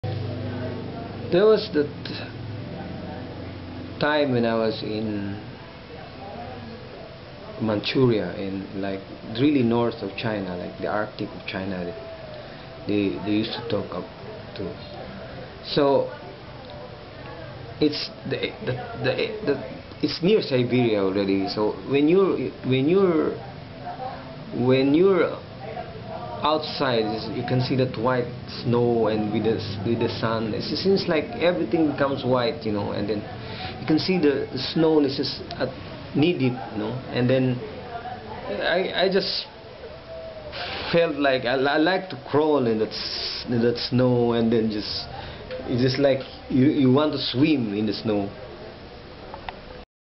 Snowing in Manchuria
the white snow of manzhouli and bright sun